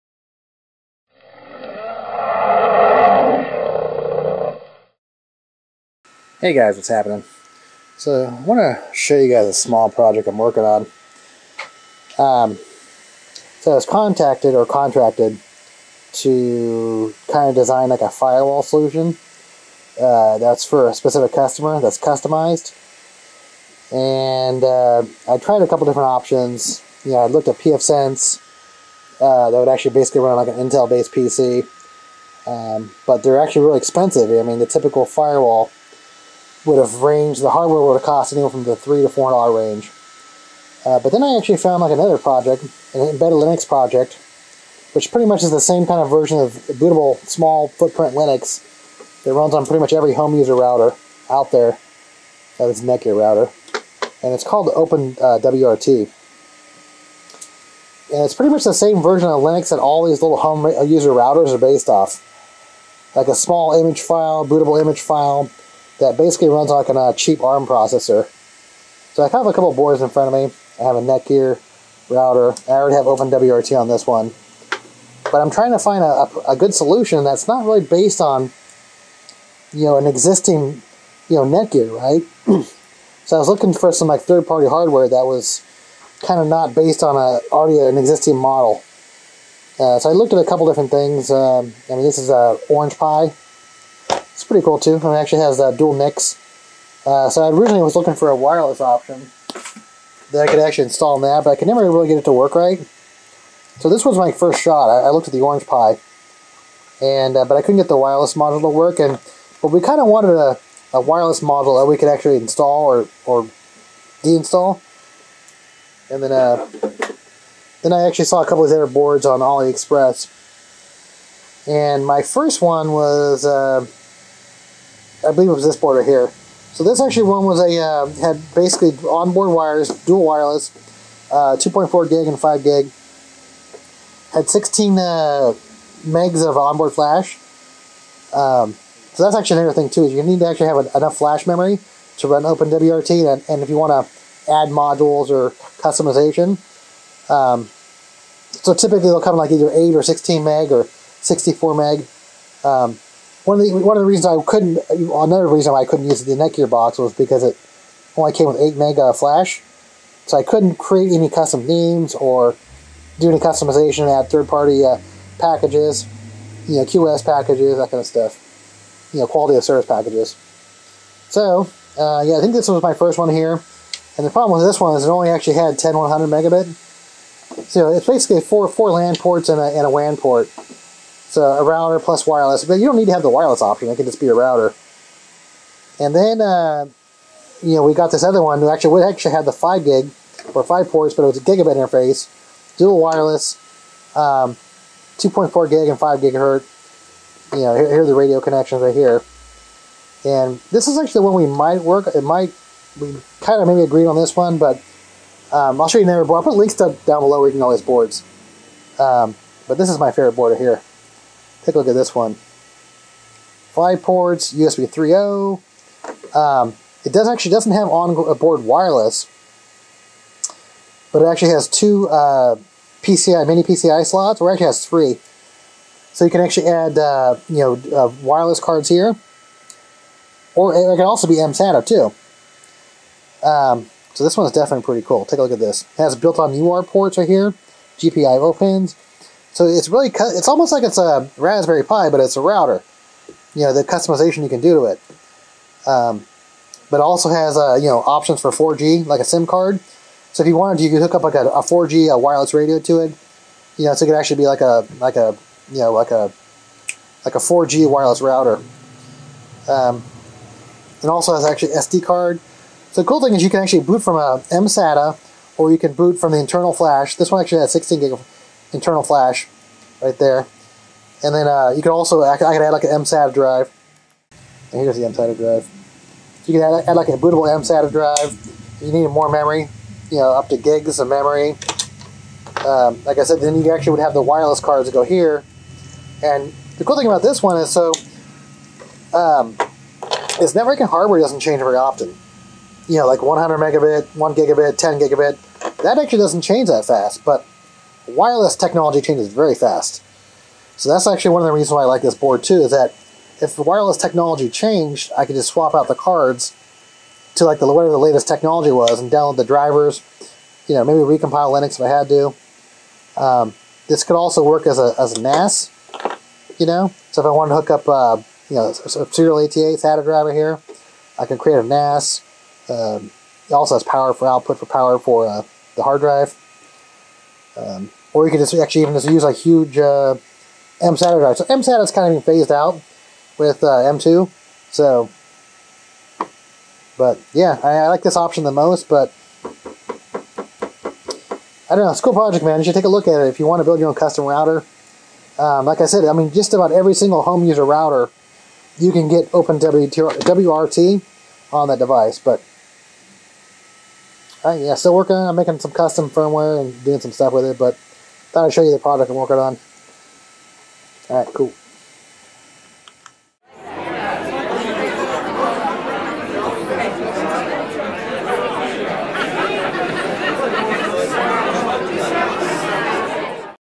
openwrt hardware selection, testing different board features. U7628-01
I was contracted to design a custom firewall based on openwrt. Openwrt is a small Linux image that usually runs on a arm processor or SOC (System On Chip). Most commercial and home user routers run on some variant of Linux.
Orange Pi R1 Plus

Older Board U7628-01
If you found this video helpful or would like help fund future experiments. please donate below.